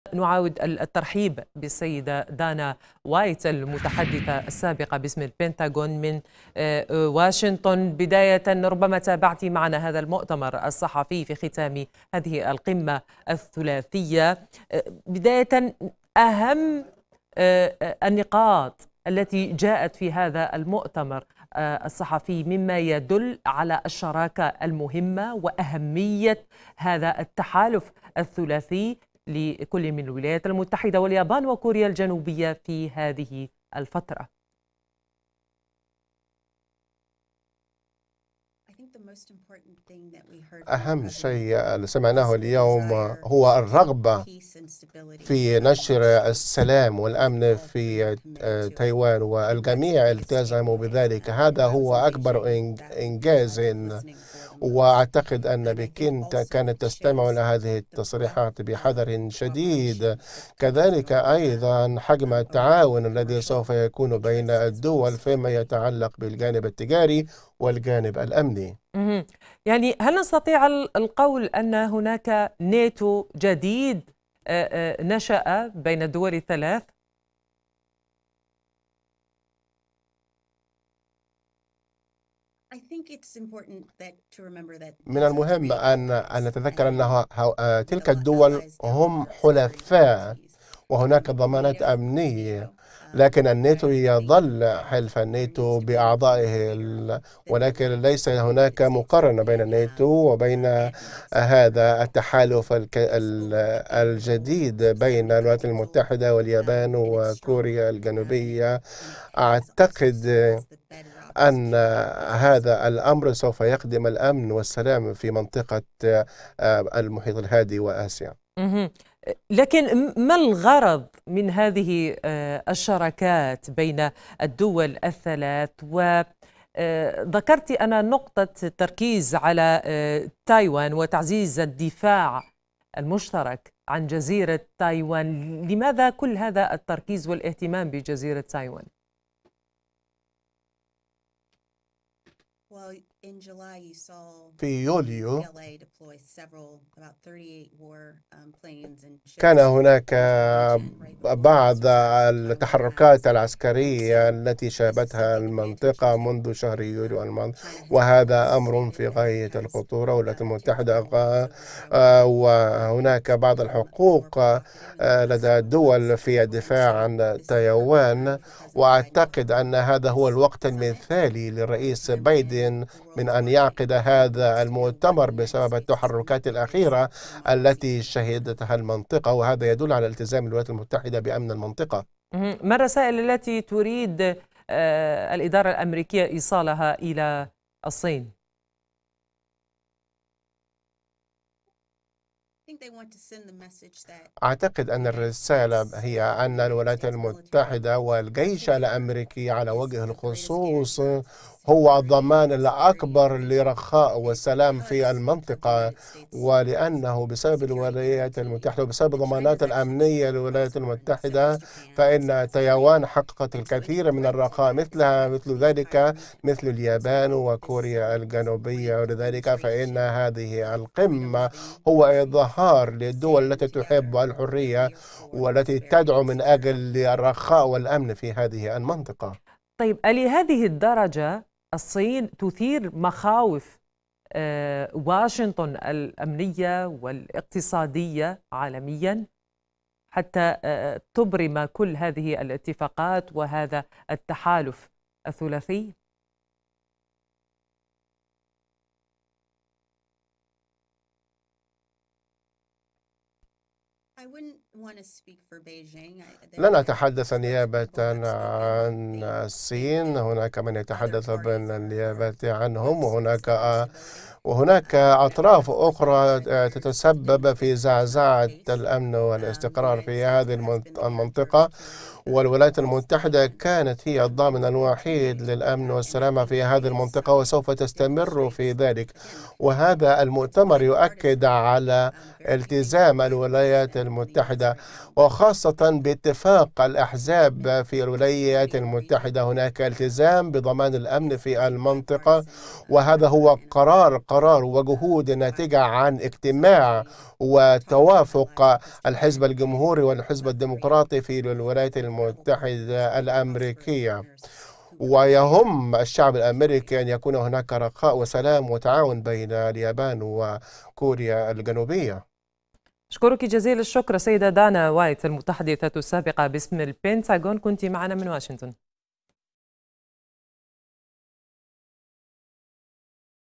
المتحدثة السابقة باسم البنتاغون: القمة الثلاثية بعثت برسالة هامة للصين يجب أن تتفهمها
عقدت في منتجع كامب ديفيد قمة ثلاثية بحضور الرئيس الأميركي جو بايدن ونظيره الكوري الجنوبي يون سوك يول ورئيس الوزراء الياباني فوميو كيشيدا. وأكد بايدن أن تعزيز التعاون يوفر الأمن للدول الثلاثة وللعالم اجمع على حد قوله. من جهته شدد مستشار الأمن القومي الأميركي جيك سوليفان على أن التعاون الأميركي الياباني الكوري الجنوبي لن يكون بمثابة ناتو جديد في منطقة المحيط الهادئ.. وللمزيد من التفاصيل تنضم الينا من واشنطن دانا وايت المتحدثة السابقة باسم البنتاغون مع الإعلامية سهام عبد القادر عبر